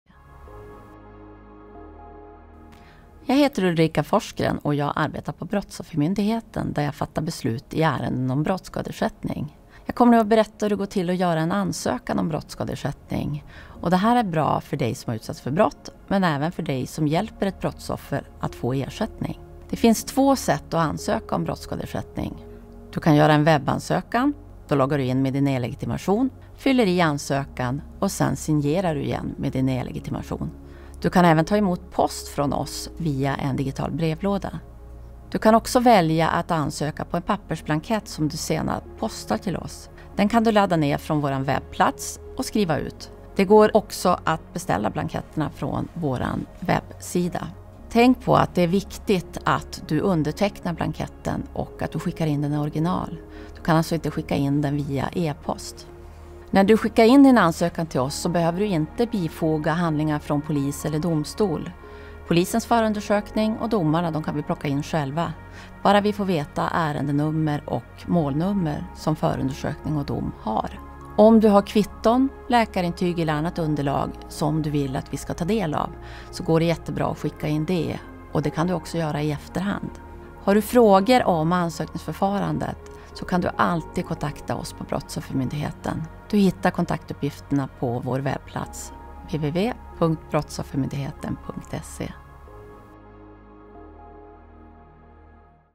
Att ansöka om brottsskadeersättning
För att kunna få brottsskadeersättning fyller du i en särskild ansökningsblankett. Du kan göra en ansökan via webben eller fylla i en blankett som du skickar via post.

Du behöver i de flesta fall inte skicka med några handlingar tillsammans med din ansökan. Polisens förundersökning och domen kan Brottsoffermyndigheten ta in. Om du har kvitton, läkarintyg eller annat underlag som du vill att vi ska ta del av går det bra att skicka in det. Skicka inte in känsliga uppgifter via e-post.